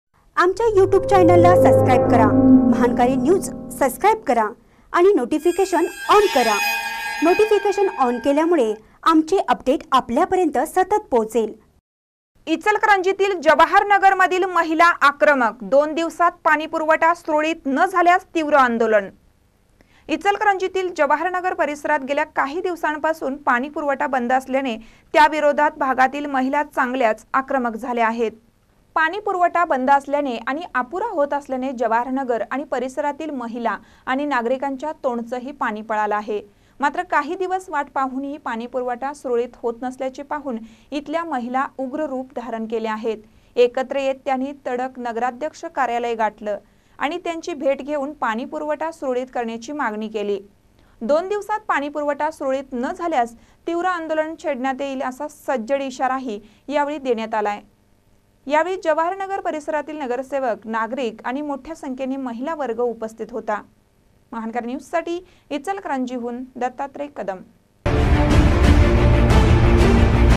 इचलकरंजीतील जवाहर नगर मधील महिला आक्रमक : दोन दिवसात पाणी पुरवठा
इचलकरंजीतील जवाहर नगर मधील महिला आक्रमक : दोन दिवसात पाणी पुरवठा सुरळीत न झाल्यास तीव्र आंदोलन